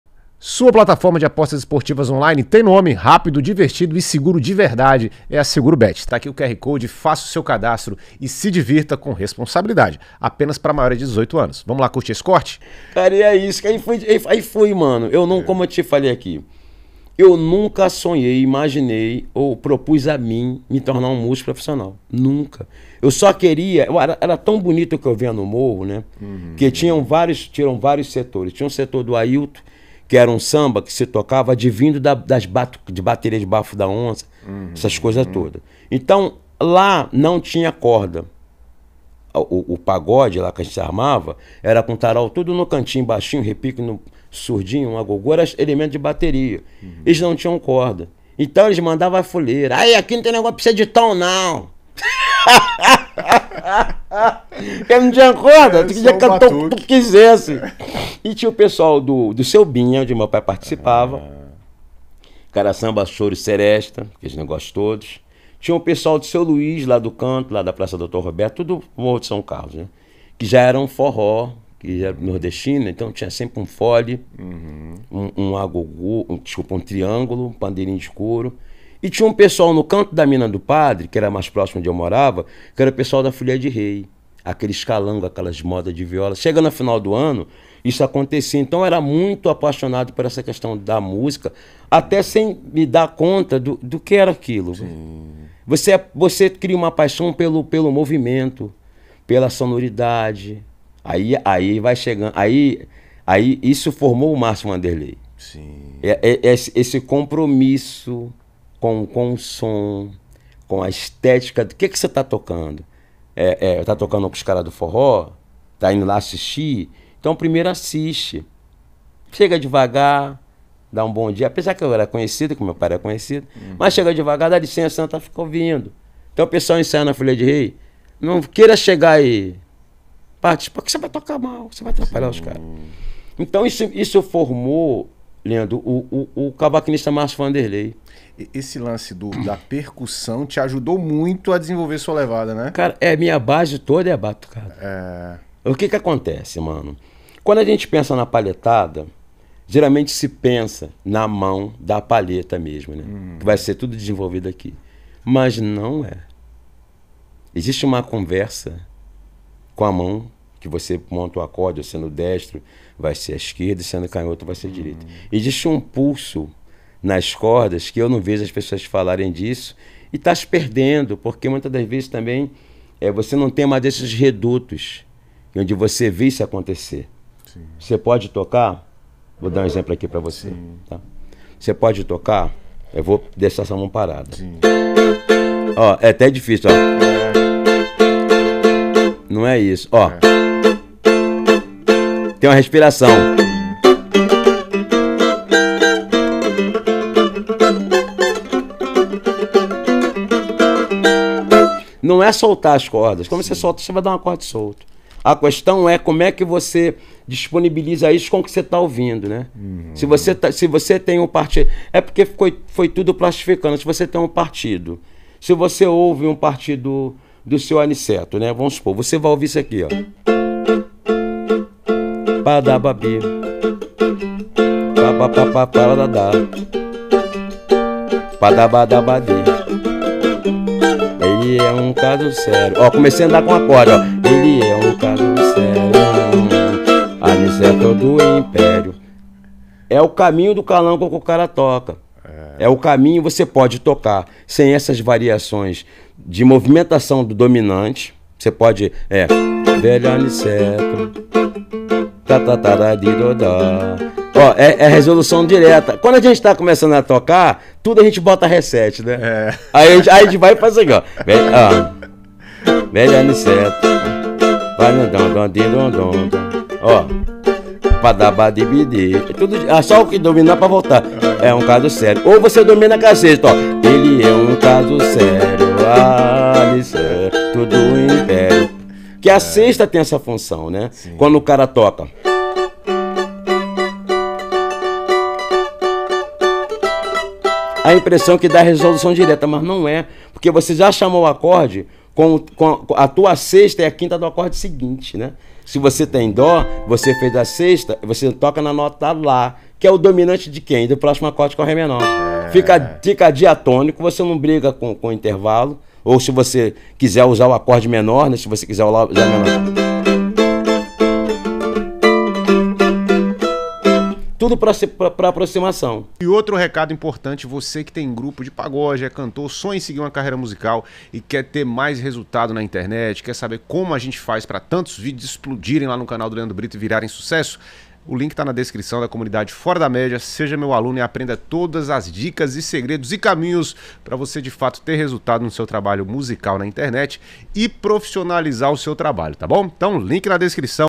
LEVADA DE CAVAQUINHO COM MÁRCIO VANDERLEI | Brito Podcast
Brito podcast com Marcio Vanderlei . Curta na integra esse super papo no canal do Brito Podcast. Link abaixo

ACESSE JÁ e aprenda todos os caminhos para o seu trabalho musical crescer no mundo digital.